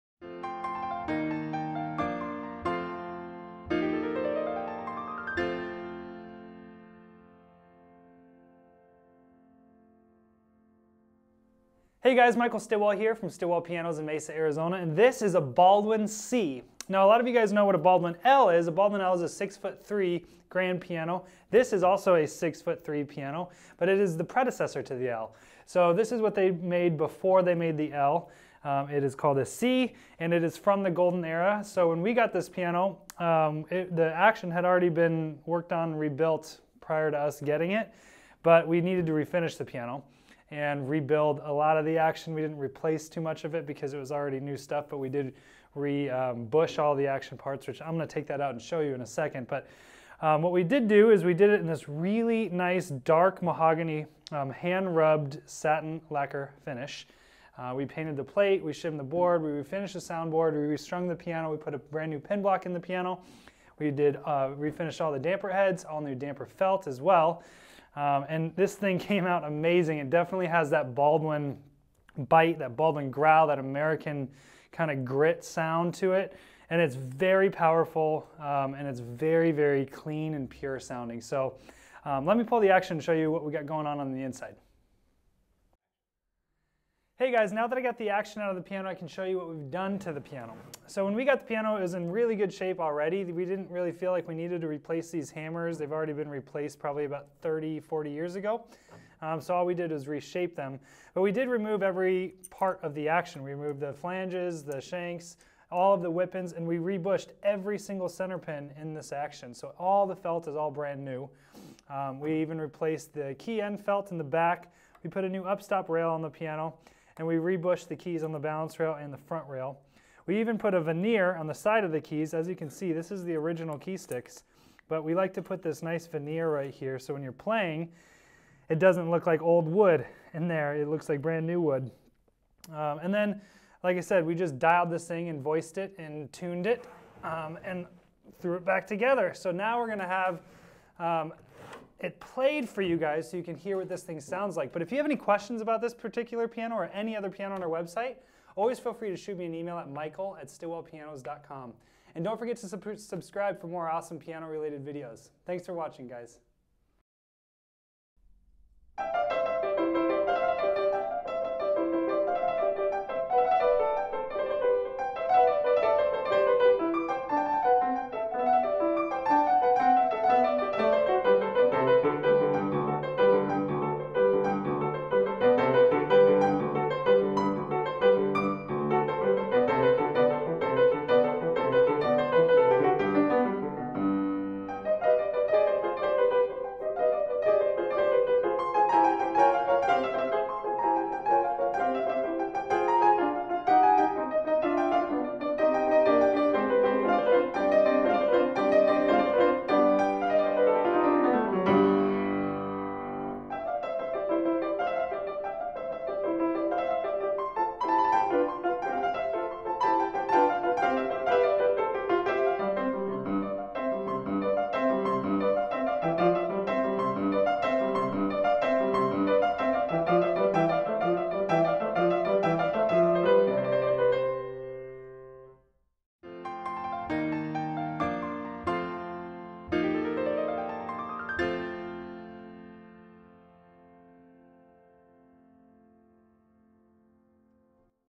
Golden Era Baldwin Piano Model C | Built in 1917
Let us take a look together on this Baldwin Piano model C. Now a lot of you guys know what a Baldwin L is, it is a six foot three grand piano, and this is also a six foot three piano. But it is the predecessor to the L Pianos. So this is what they made before they made the Baldwin L, and it is called Baldwin piano model C. it is a golden era piano that was built in 1917.
We refinished the piano and rebuilt a lot of the action. We didn't replace too much of it because it was already new stuff, but we did re-bush all the action parts. What we did do is we did it in this really nice dark mahogany hand rubbed satin lacquer finish, we painted the plate, shimmed the board, finished the sound board, strung the piano, put a brand new pin block in the piano, and we did refinished all the damper heads. This Baldwin C piano came out amazing!
It definitely has that Baldwin bite... that bulb and growl that American kind of grit sound to it and it's very powerful, and it's very, very clean and pure sounding.